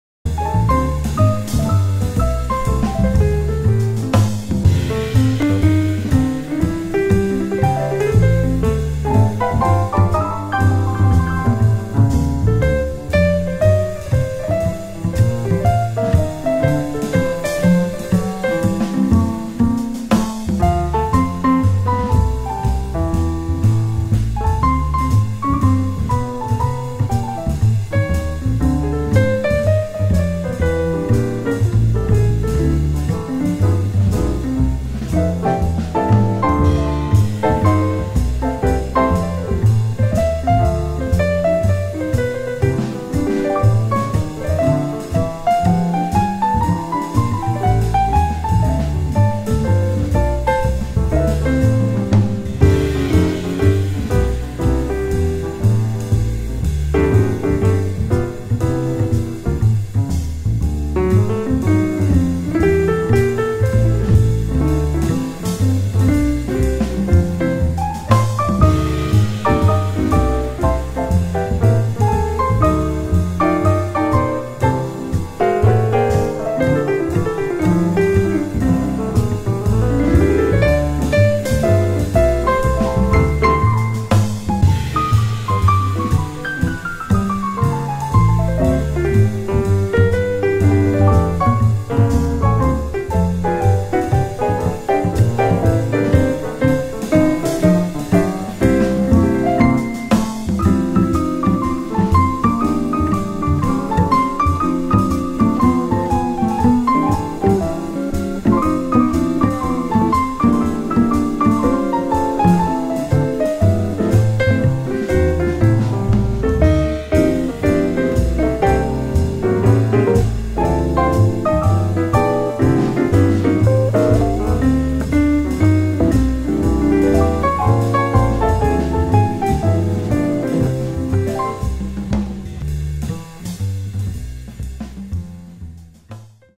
Little expromt in stile swing by Yuriy Reshetar
A short jazz improvisation by Yuriy Reshetar.
Keyboard used:Kurzweil sp4-8.